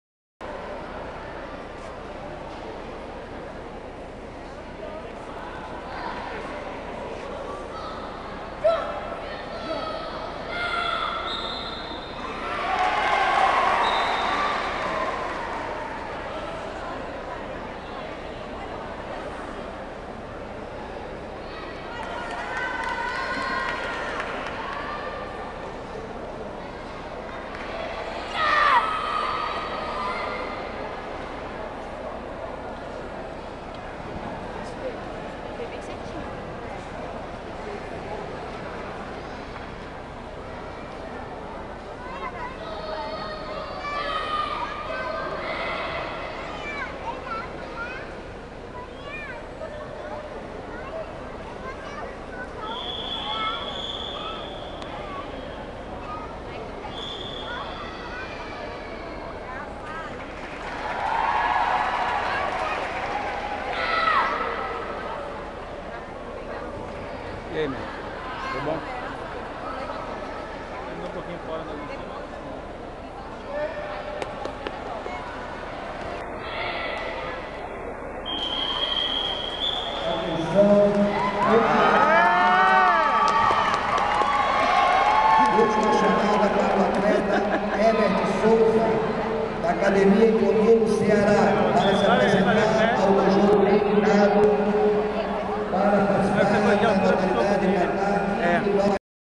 ALEX BRUNO XXII CAMPEONATO NORTE NORDESTE DE KARATÊ-DÔ TRADICIONAL, UFRN RIO GRANDE DO NORTE NATAL
ALEX BRUNO CAMPEÃO MIRIM ATÉ 9 ANOS KATÁ INDIVIDUAL E COMITÊ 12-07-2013